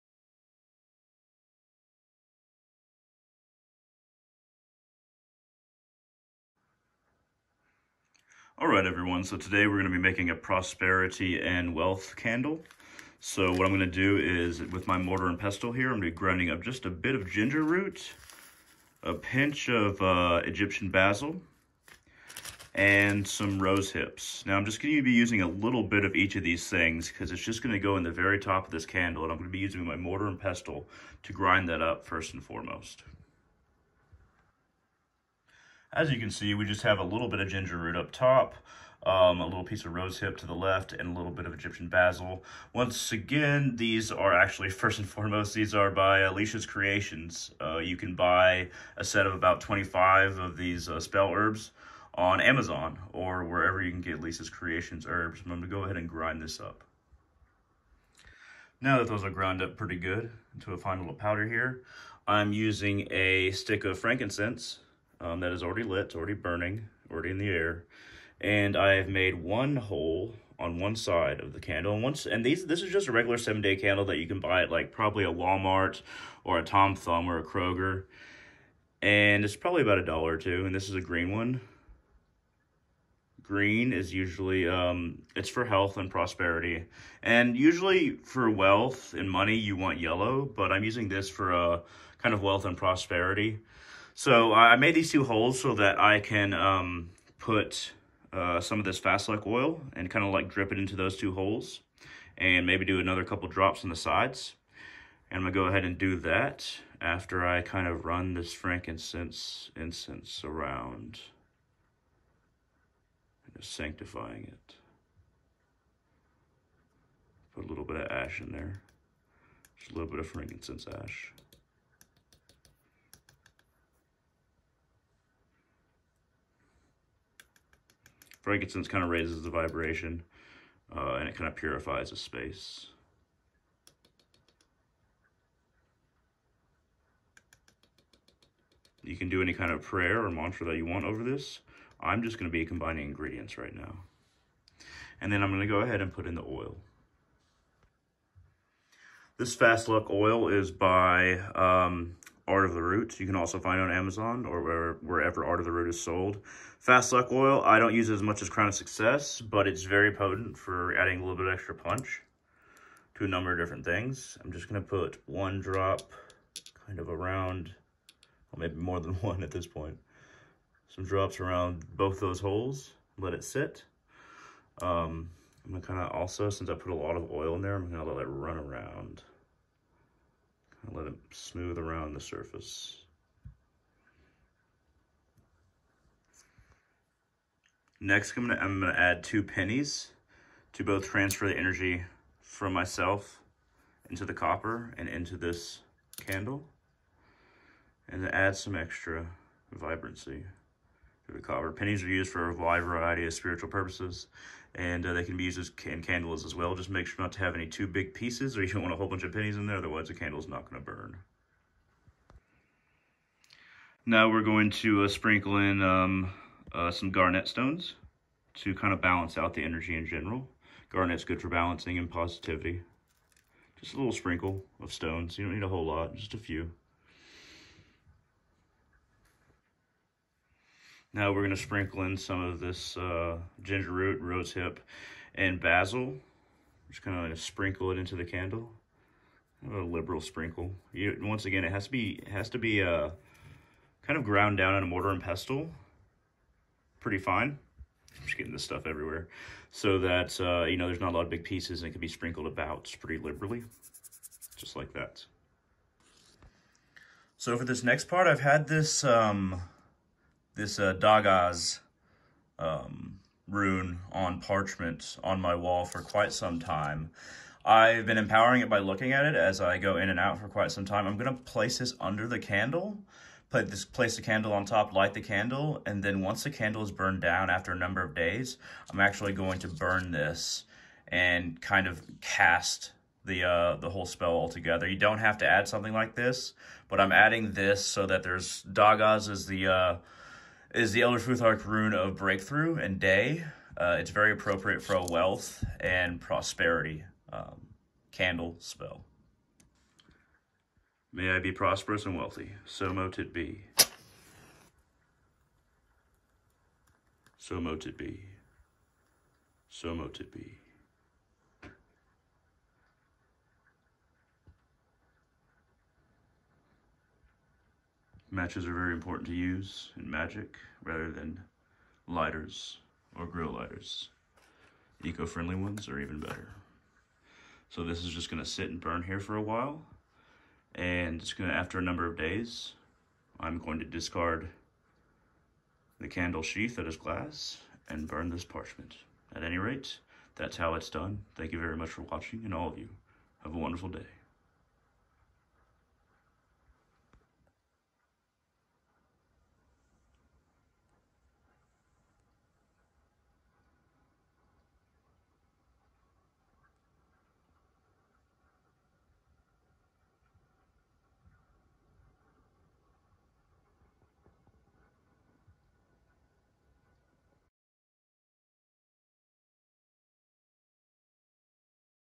Prosperity and Wealth Candle
This is the making of a prosperity and wealth candle of my own concoction! I hope this helps!